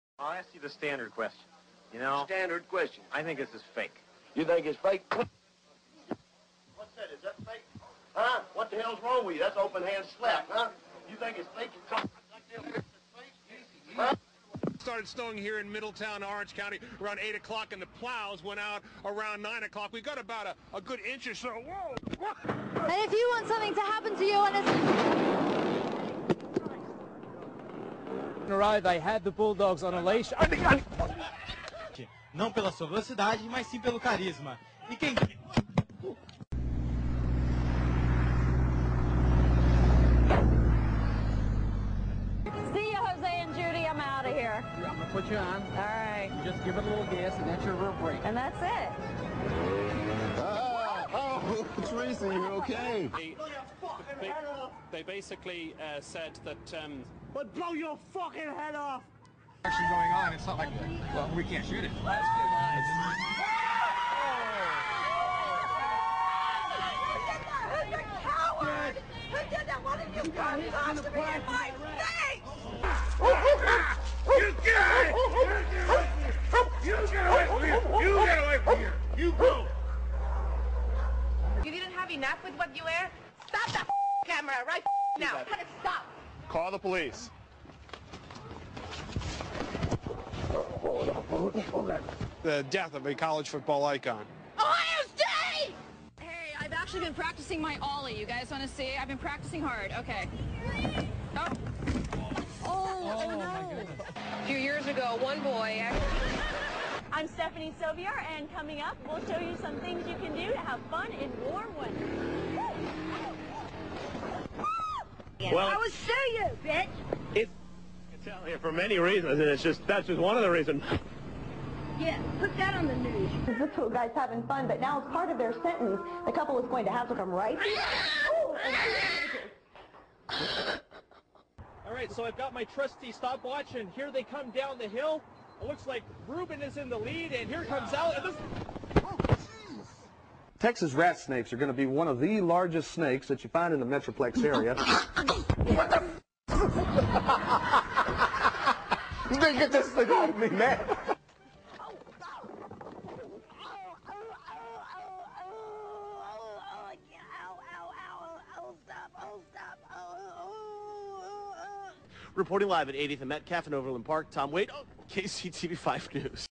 Reporters getting hurt bad! bloopers!
funny clips of reporters geting owned